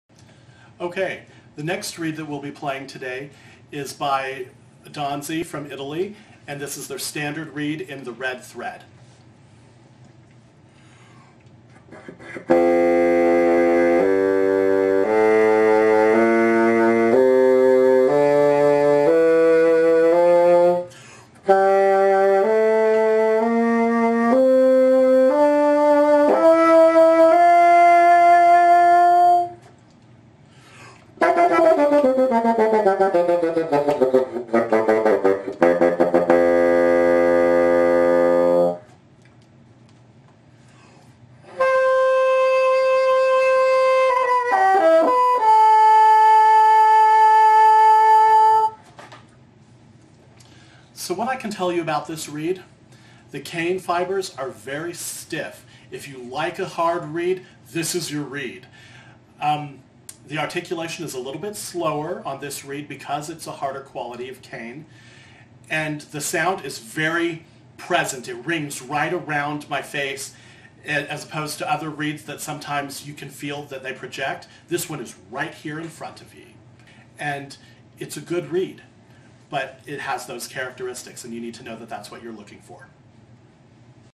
Danzi Professional Bassoon Reed Demo
K.C. Chai of Hodge Products, Inc. is playing and demonstrating the Danzi Professional reed with Red thread. It is made of Danzi cane (harder) and shaped on Danzi's American Industrial shaper.